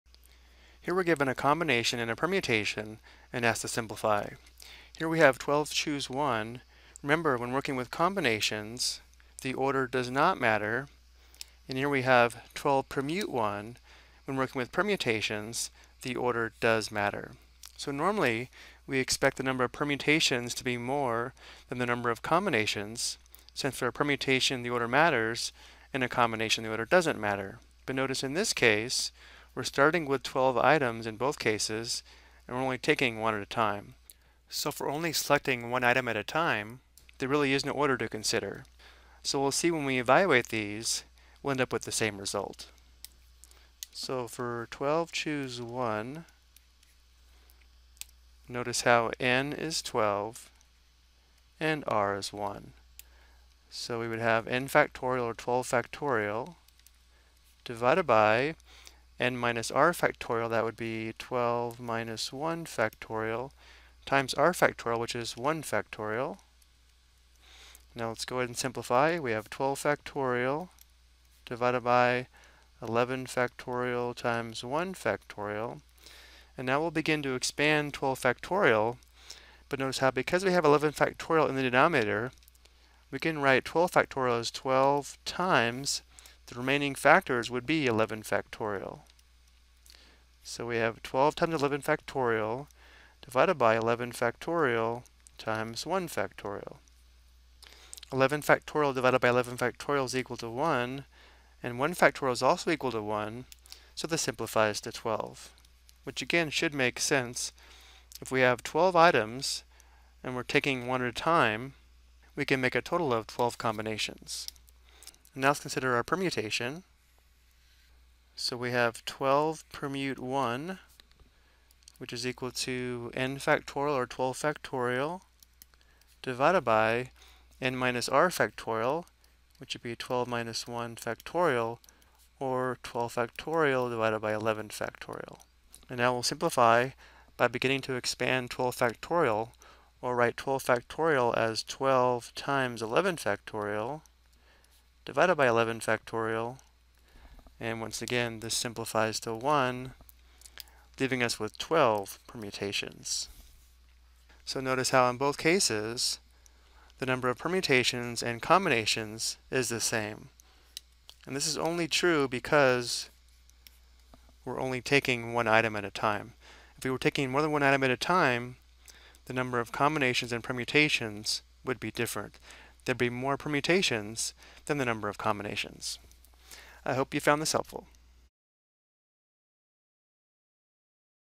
Ex: Evaluate a Combination and a Permutation - (n,1)
This video explains how to evaluate a combination and a permutation with the same value of n and r = 1.